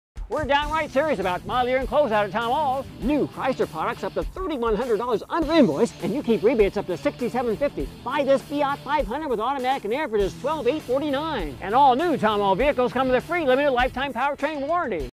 New FIATs Deals | Fort Wayne Area FIAT Dealer
Our FIAT dealership is having incredible close out sales that Fort Wayne area drivers need to take advantage of. For instance, this 2017 FIAT 500 is brand new and is at an all-time low price. Come visit our dealership and we'll set you up with a test drive with it.